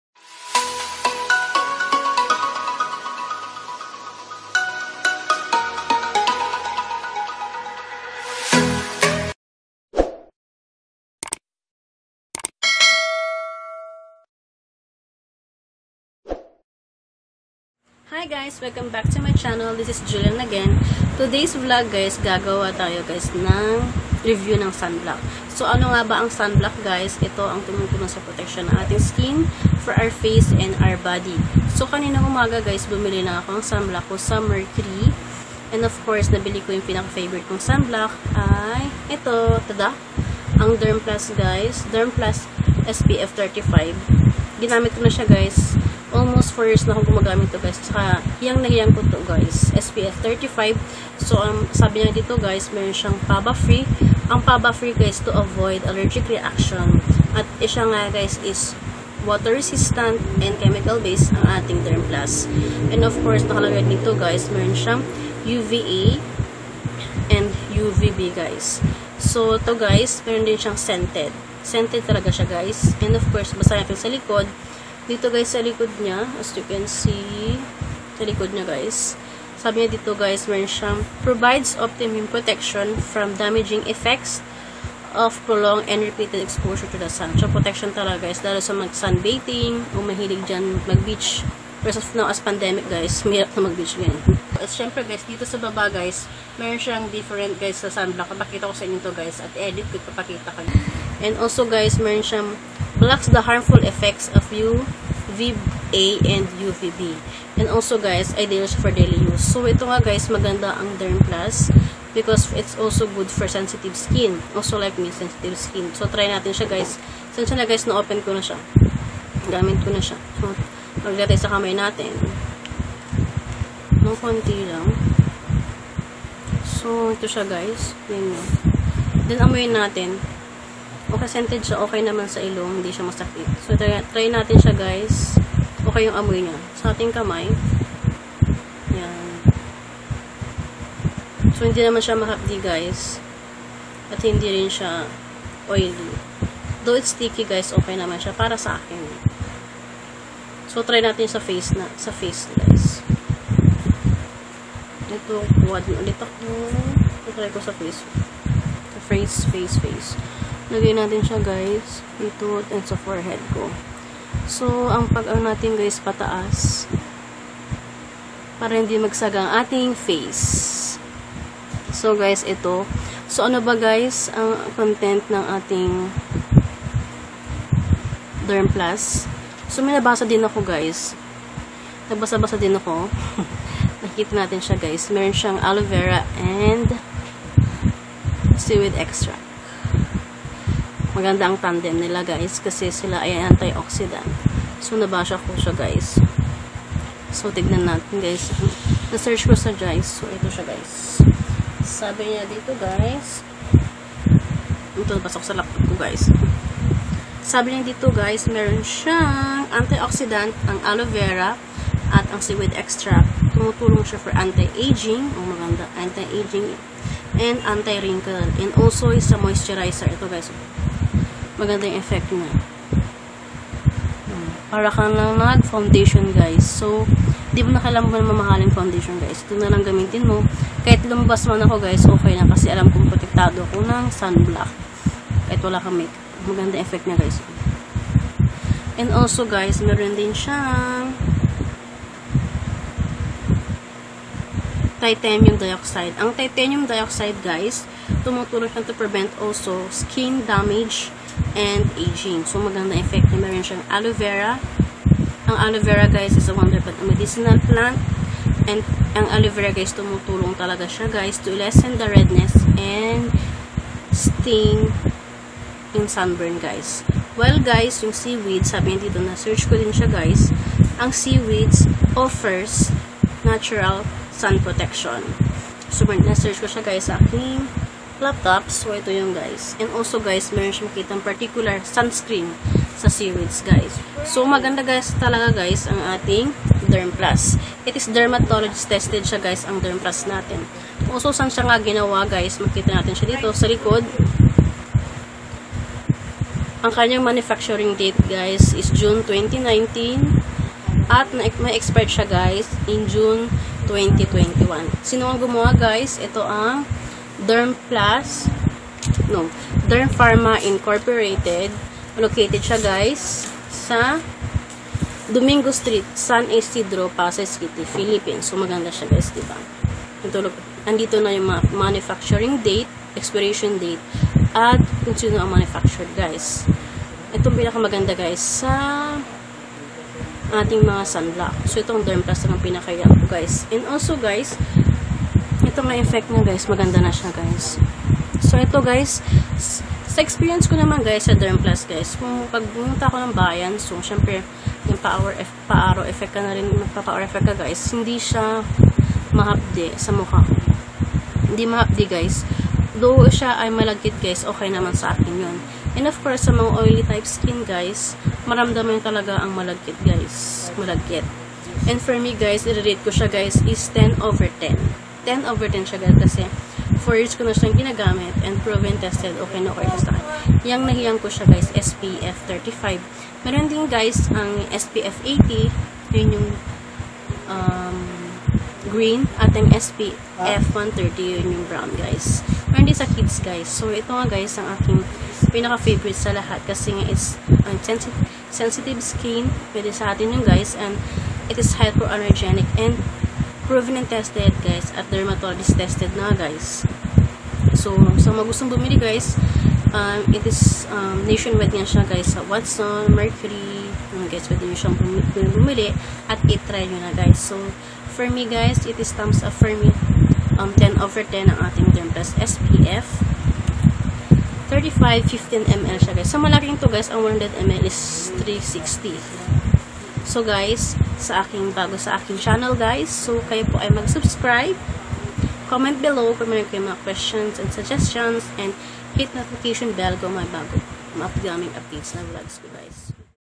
DERMPLUS SPF 35 SUNBLOCK REVIEW | Honest Review
Hello, guys! Hope you're doing well.

This was my second time to review a beauty product.

Wastons and Mercury Nationwide and other leading drug stores.
Dermplus SPF 35 15ml- 59 pesos on Mercury.

Thank you, guys. God bless us all and stay healthy :)